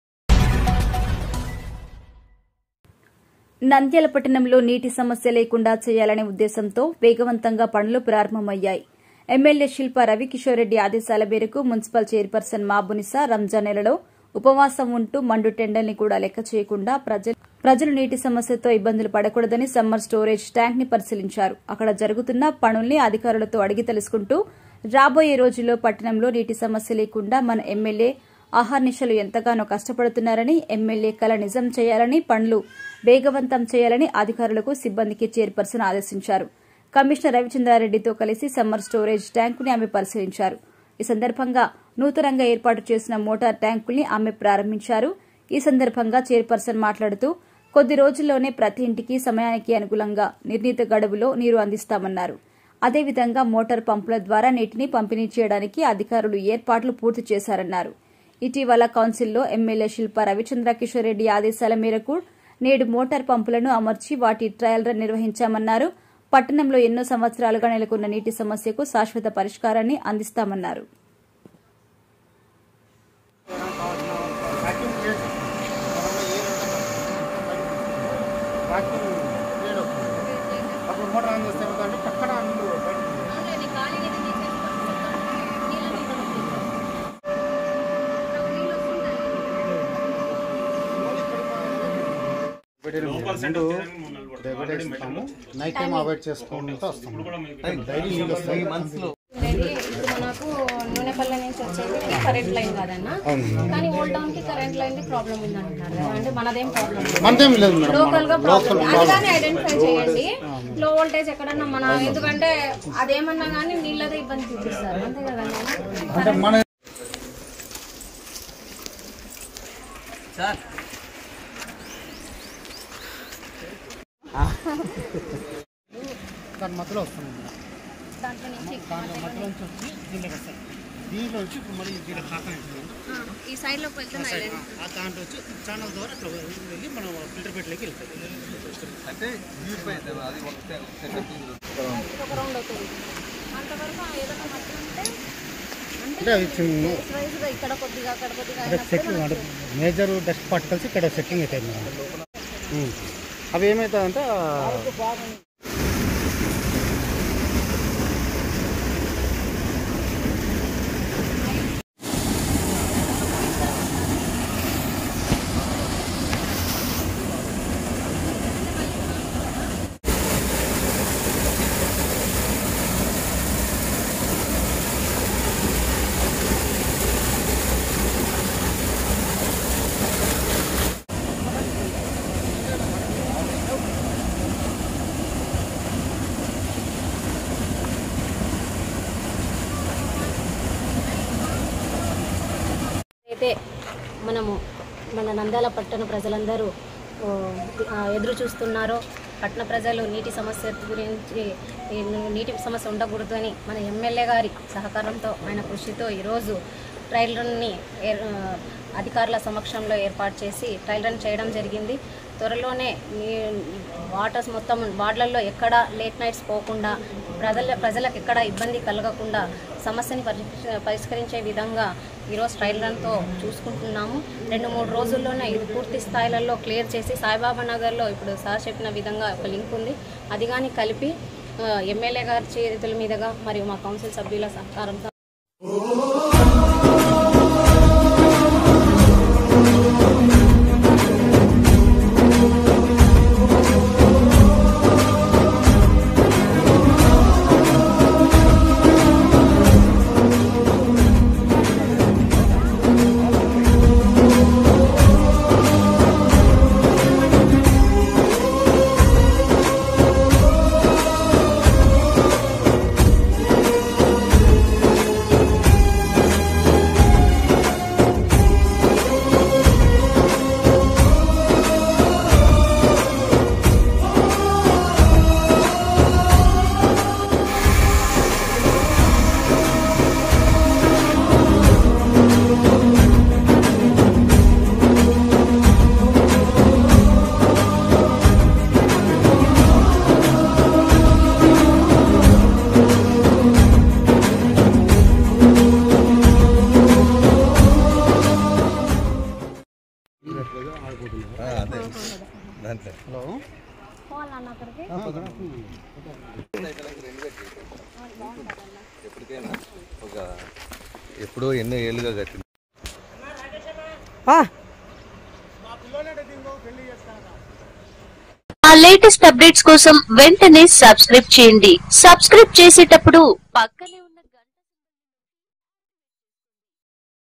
నంద్యాల ప్రజలకు నీటి సమస్యకు.మండు ఎండలలో చైర్ పర్సన్..!nandyal news9
Nandyal News9 నంద్యాల పట్టణంలో నీటి సమస్య లేకుండా చేయాలని ఉద్దేశంతో వేగవంతంగా పనులు ప్రారంభమయ్యాయి ఎమ్మెల్యే శిల్ప రవి కిషోర్ రెడ్డి ఆదేశాల మేరకు మున్సిపల్ చైర్ పర్సన్ మాబునిస